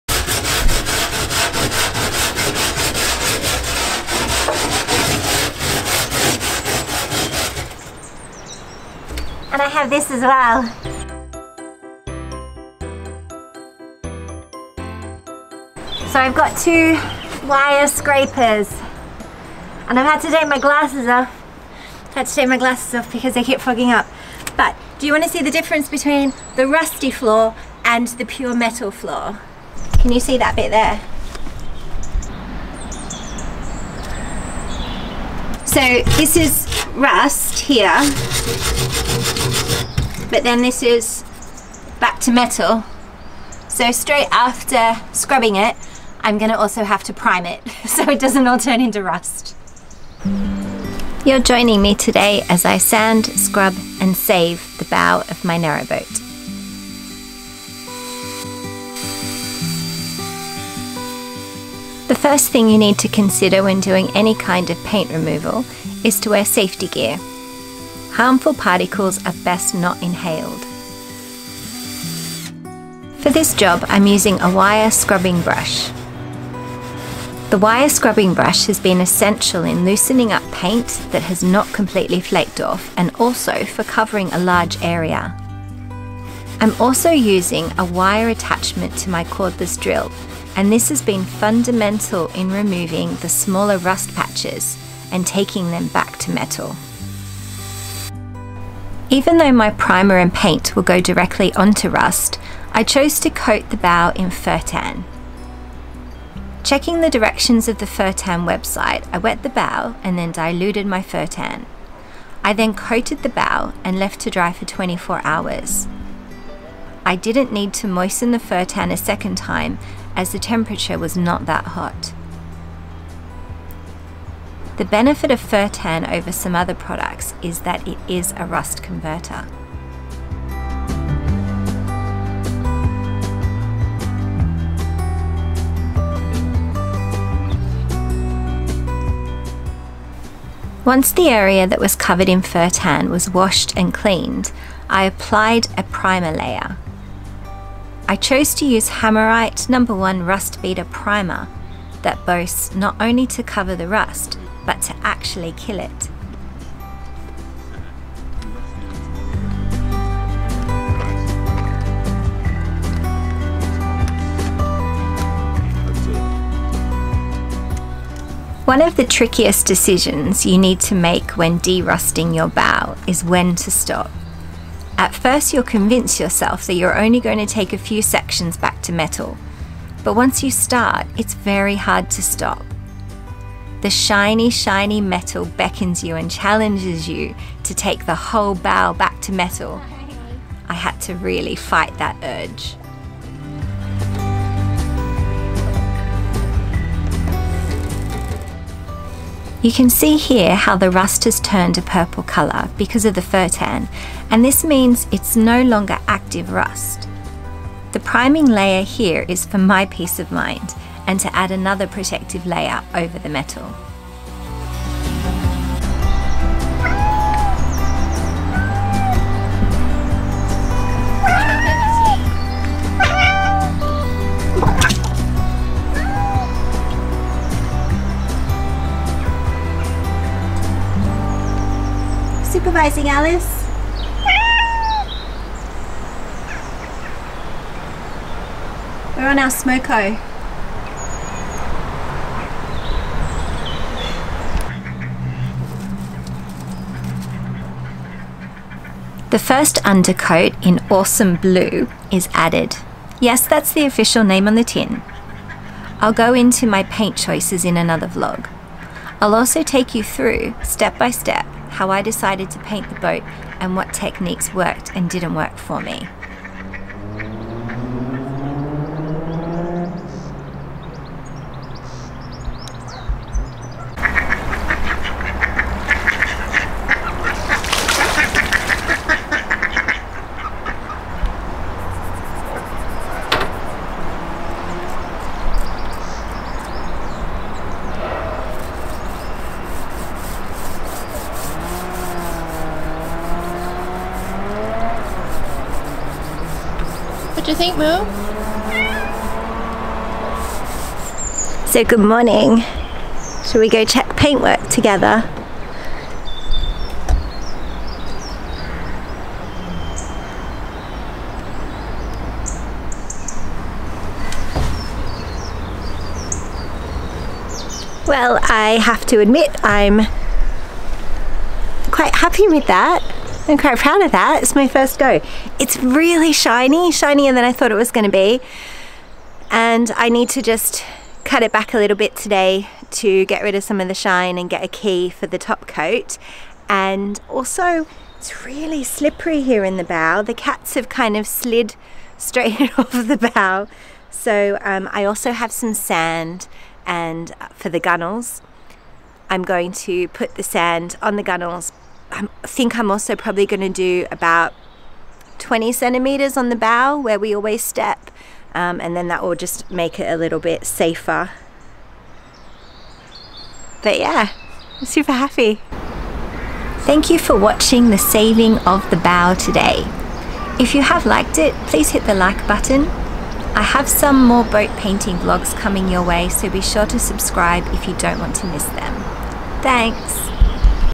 Rescuing the bow of my narrowboat from rust
The bow of the narrowboat needed some attention as it had become rusty. In this episode I show you how I dealt with the rust and also give you a preview of the new boat colour when I paint the bow.